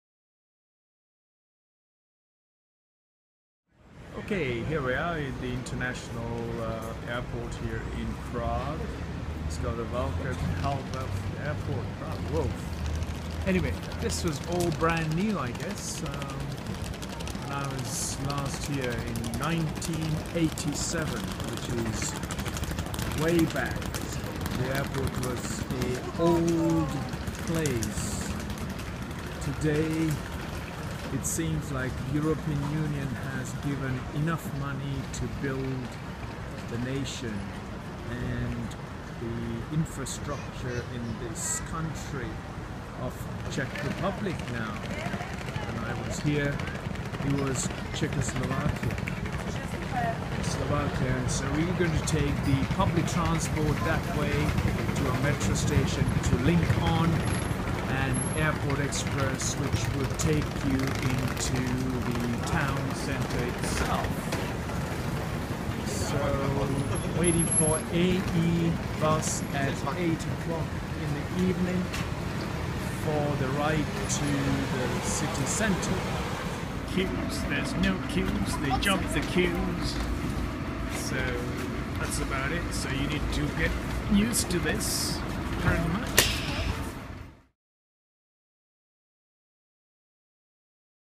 Prague Airport to City Centre
The Prague airport is located about 15 km/9 miles from the city center. It takes approximately 25 - 30 ...

There are direct by bus and Airport Express, Airport bus and Metro option too.